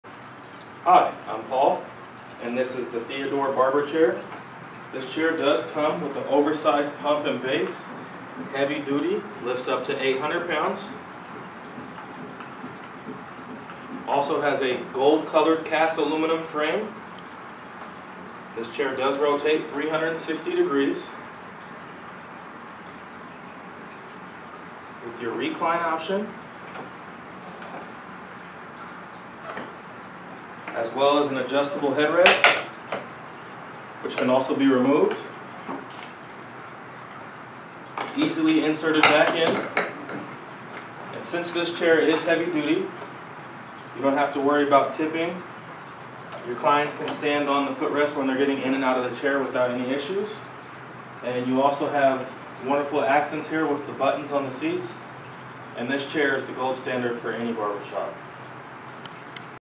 "THEODORE" Golden Barber Chair by AGS Beauty, Barber Chairs with Golden Frame
"THEODORE" Golden Barber Chair (SKU: A-902)

- 1-year warranty on the chair, 2-year warranty on the hydraulic base
- Top grade vinyl feels like real leather
- Available in 2 colours: Premium Black and Cardinal Red
- Cushions made of high density memory foam for extra comfort and longer duration of use
- The headrest is adjustable and removable
- Cast aluminium frame, reclining handle and footrest
- Polished golden plating finish
- Heavy duty hydraulic pump with white cover (Max weight allowed: 800 lbs)
- Over-sized chrome base
- Able to swivel 360 degrees
- This chair weighs about 200 lbs
- Foam and upholstery meet United States fire retardant standards

golden barber chair, gold barber chair, golden barber chairs, barber chairs in gold